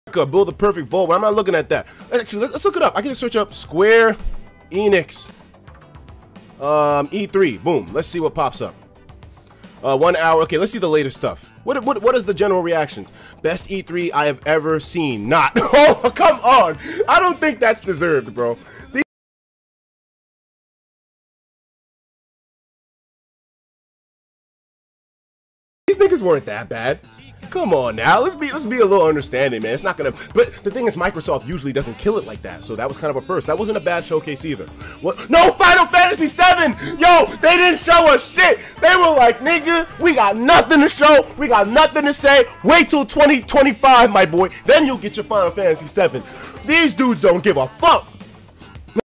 Etika responds to my comment about no FFVII during E3 2018 (Short Clip)
Etika is a YouTuber who you may recognise from that one guy reacting to stuff, not really anything big just felt nice to get noticed by someone who I like watching sometimes haha.